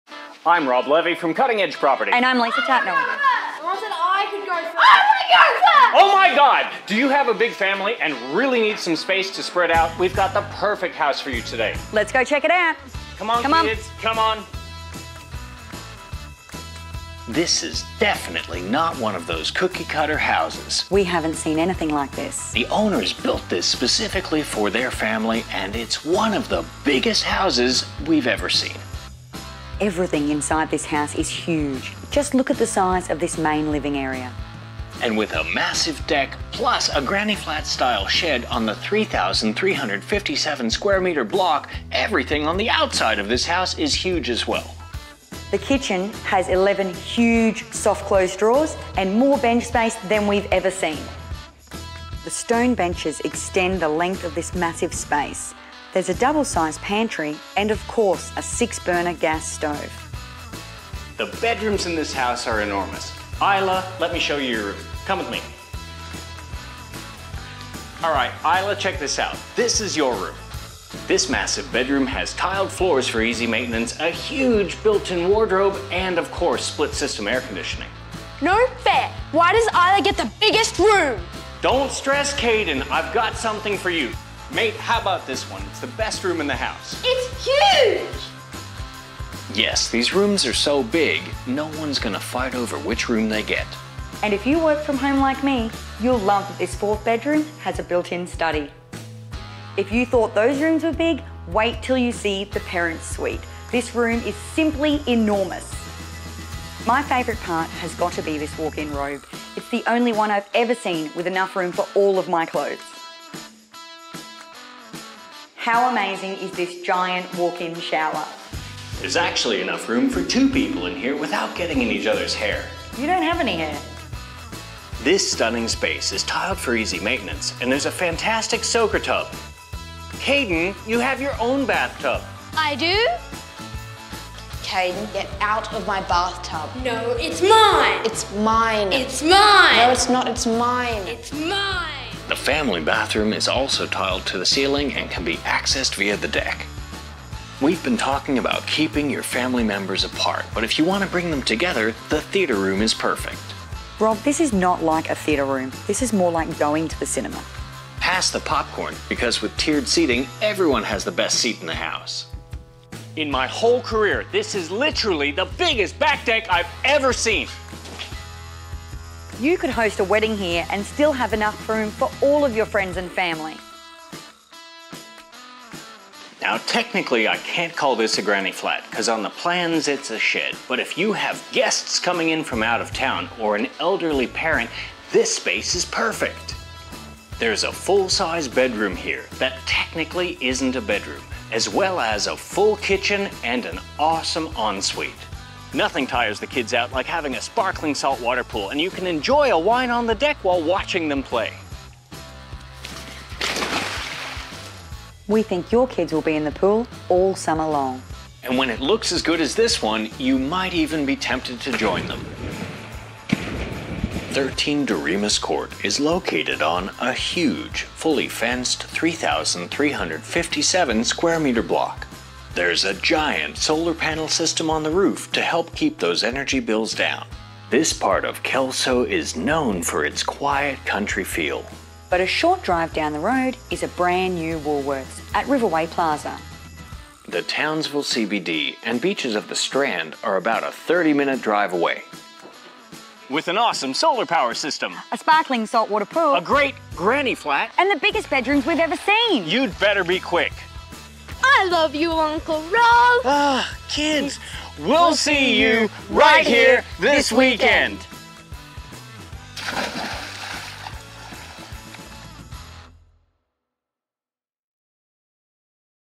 13 Doriemus Court. Kelso
🤩 There's over 4️⃣0️⃣0️⃣m² under roof here! HUGE 🍿 Movie Theatre.. 👨‍👩‍👧‍👦 Giant Bedrooms.. 🛌 Built-in shed with kitchen 👨‍🍳 great for guests! 🏊 Pool and 🌞 8kw Solar! 0ver 3000m² Block! Be Quick! 🏃‍♂️💨